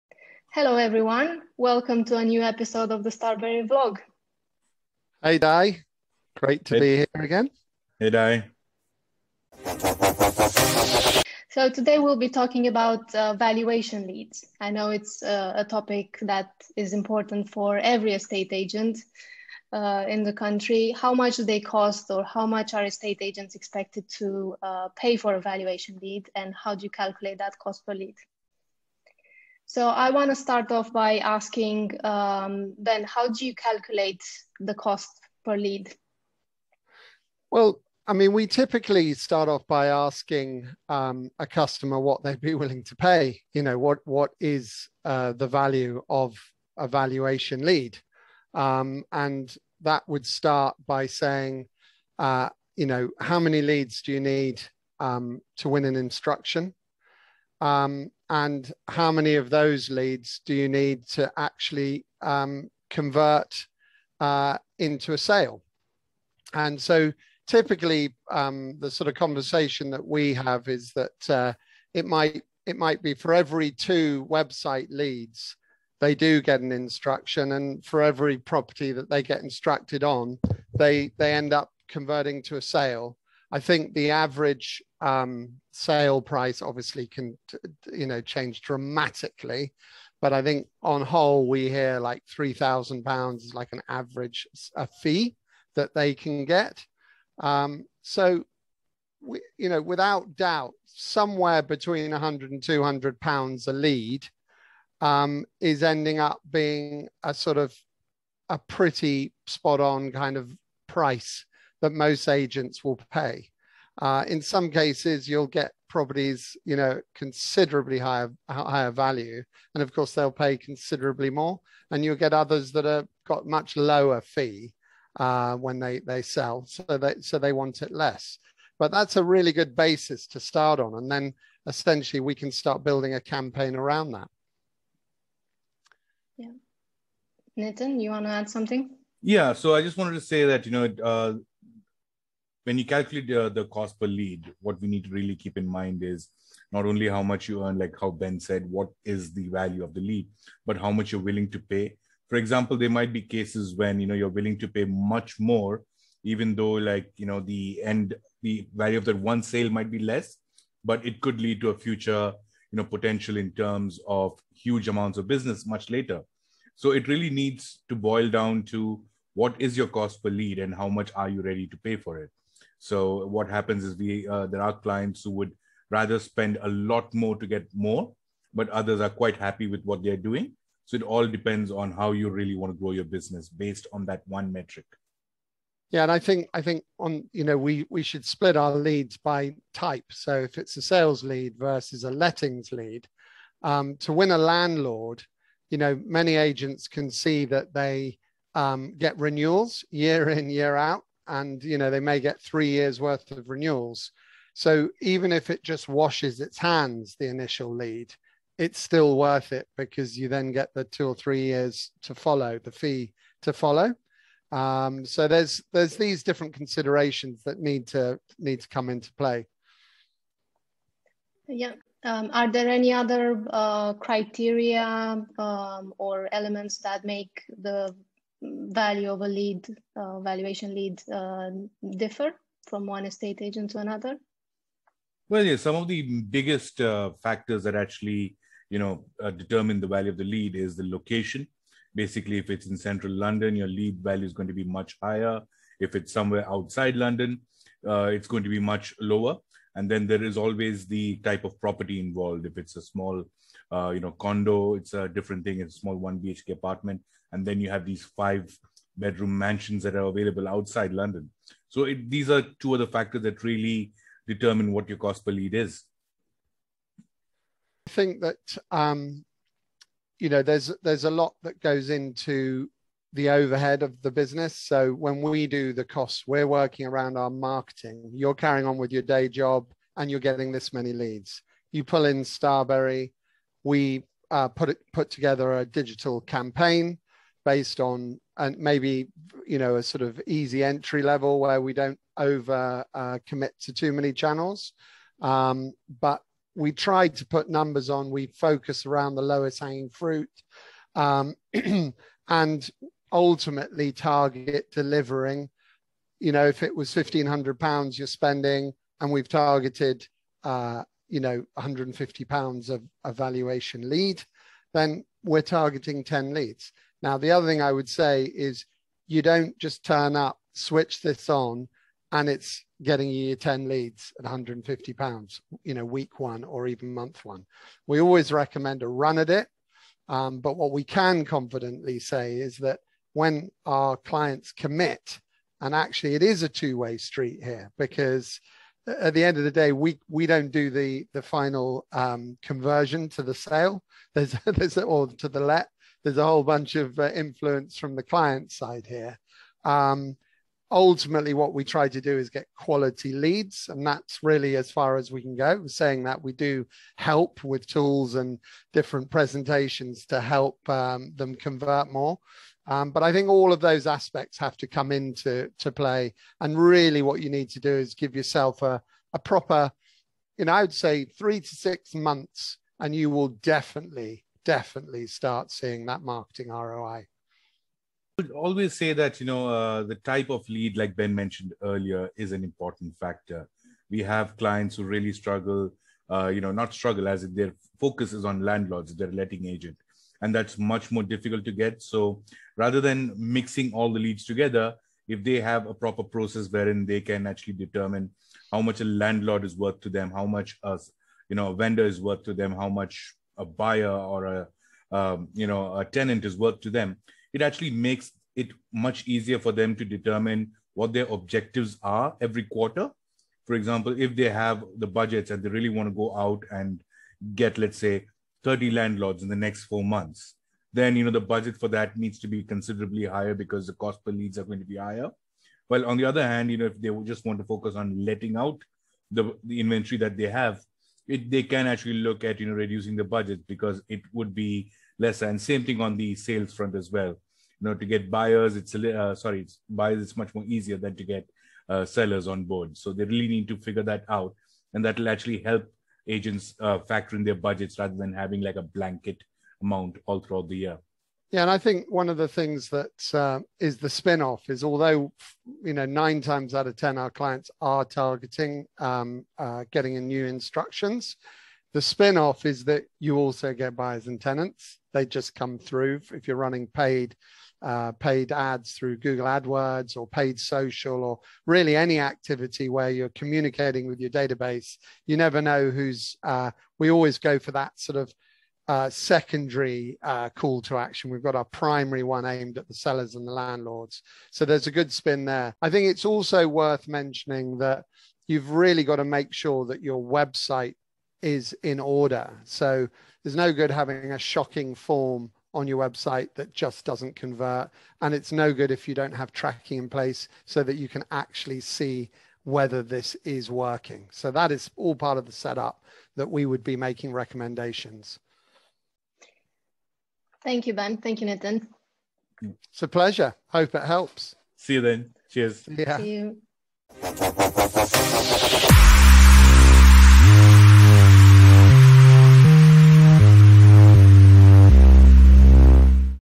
Estate Agency Valuation Leads - How much should you pay for them?
In this episode of the Starberry vlog, we cover the topic of valuation leads and how much are estate agents expected to pay for them.

Watch an insightful discussion with Ben and Nitten on how to calculate the cost per valuation lead, how this cost varies from one estate agent to another and how we can help you optimise that cost per lead with our digital marketing campaigns.

Want to know more? We're happy to help, just book a meeting with our team to discuss your lead generation requirements